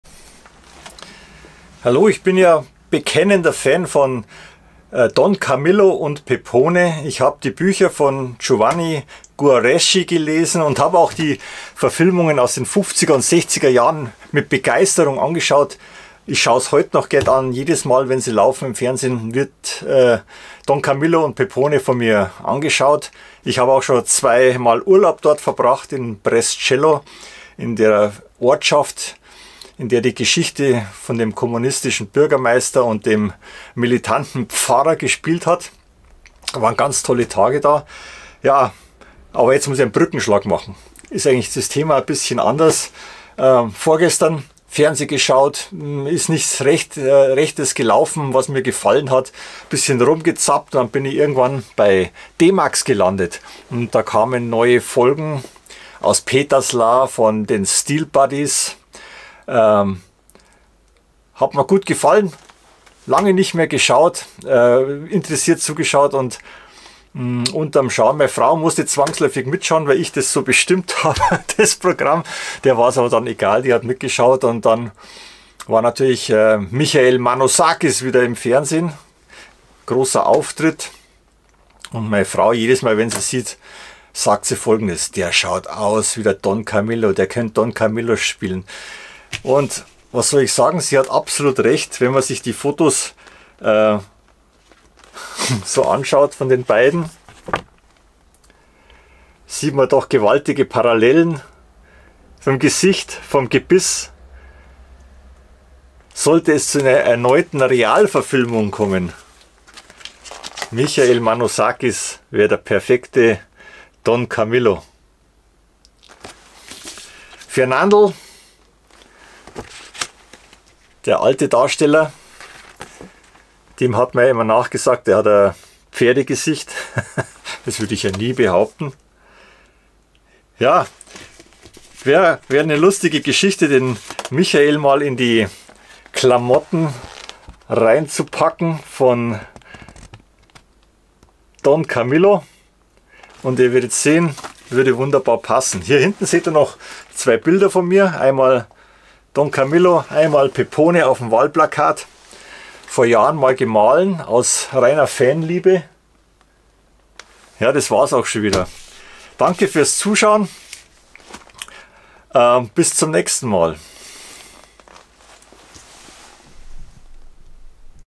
Manousakis sieht aus wie Don Camillo
Wenn man das Gesicht von Michael und Fernandel vergleicht, sieht man eine verblüffende Ähnlichkeit. Er wäre der perfekte Darsteller bei einer neuen Verfilmung von "Don Camillo und Peppone"